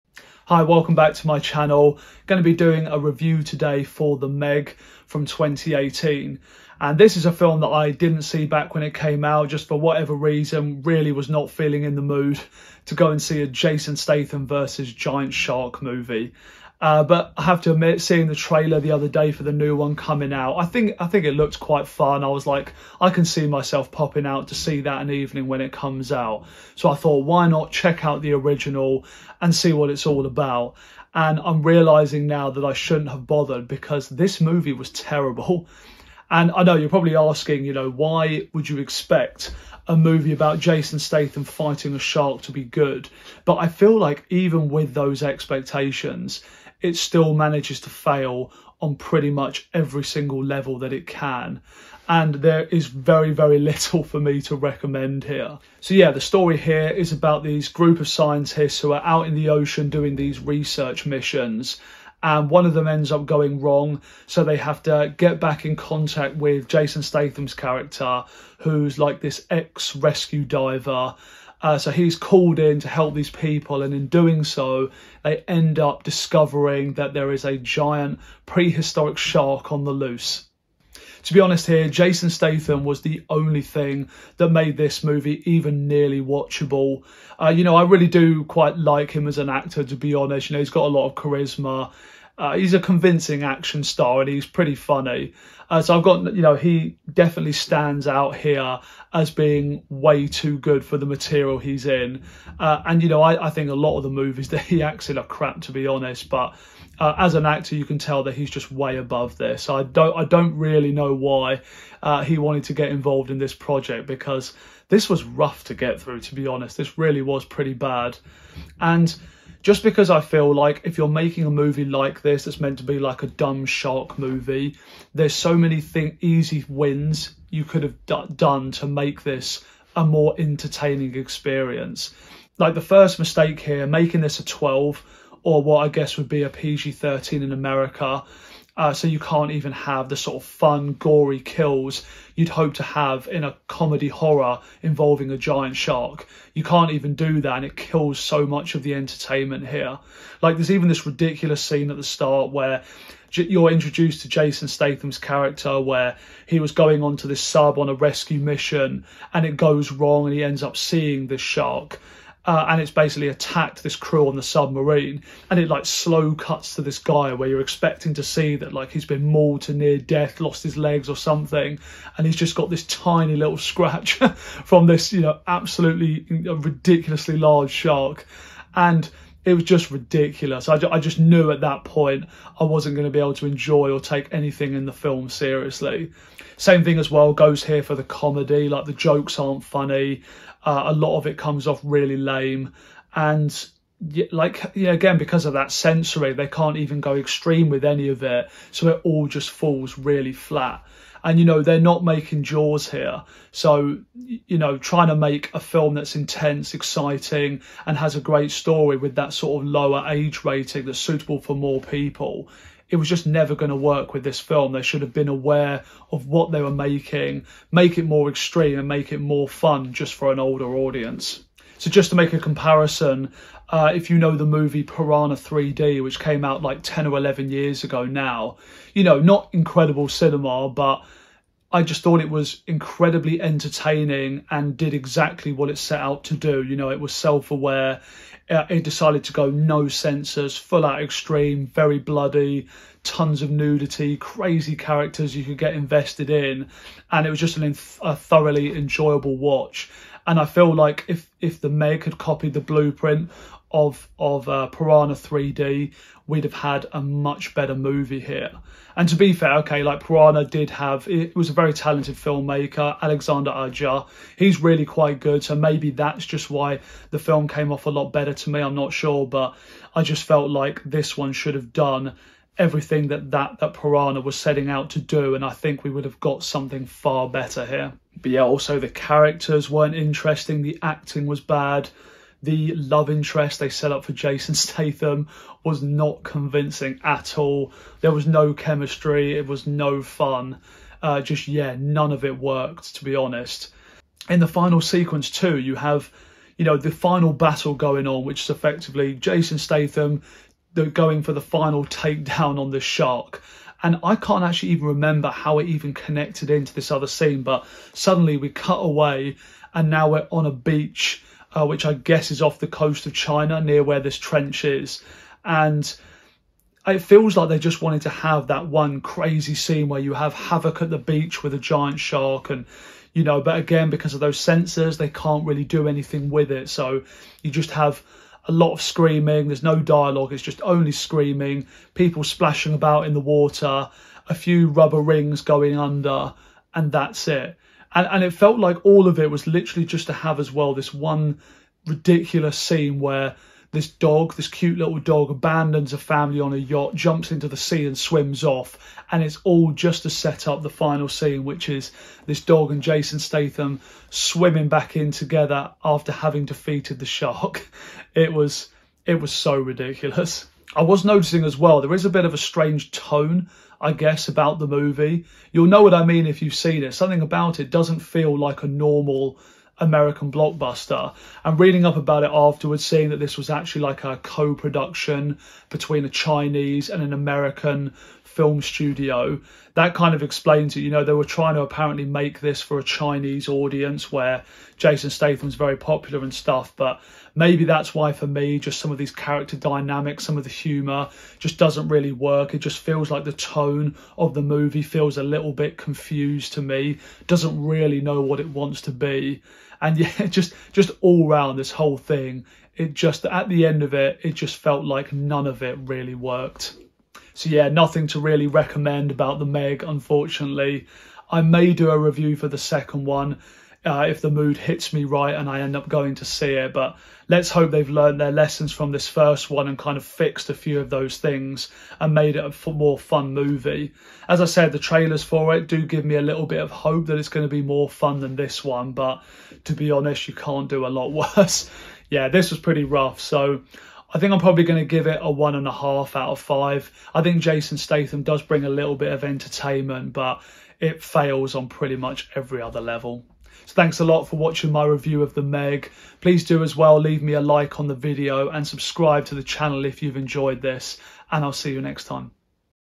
The Meg (2018) - Movie Review
Here's my thoughts on The Meg from 2018. Let's just say I hope the sequel coming out next week is a lot better!